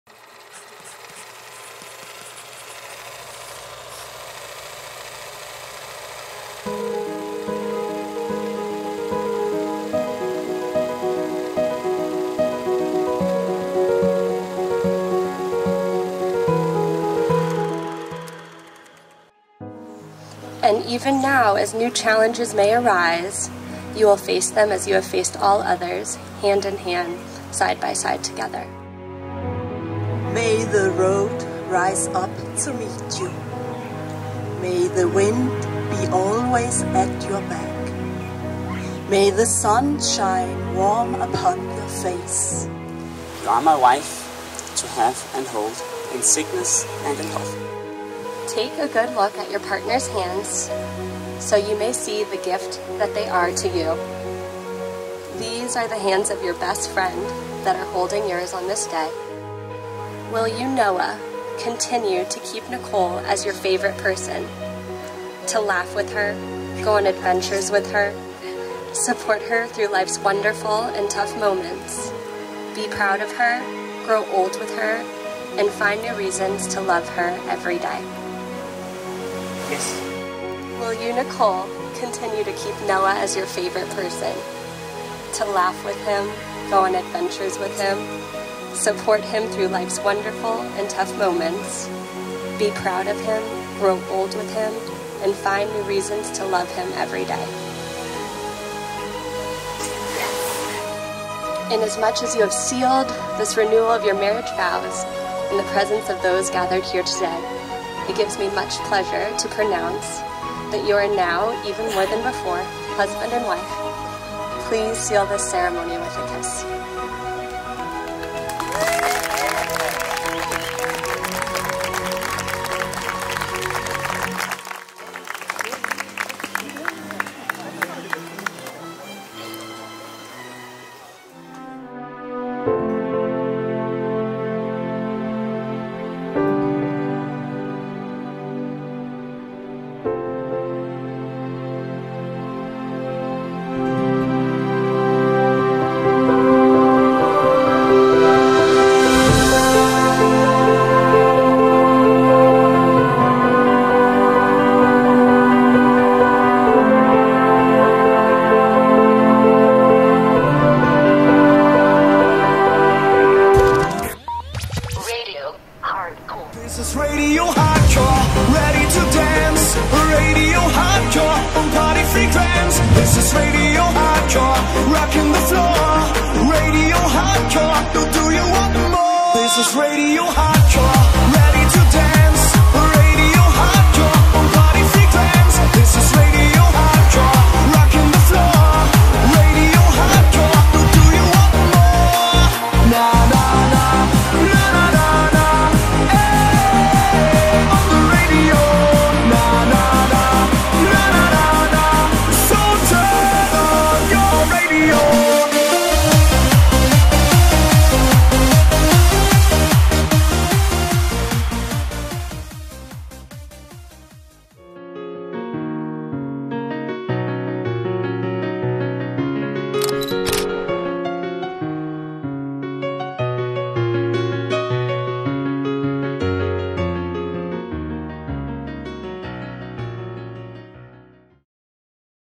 Nicole and Noah's Wedding - A Guests Perspective | Cinematic (Sony A7III)
A short cinematic film from Nicole and Noah's wedding in July - with some vintage vibes!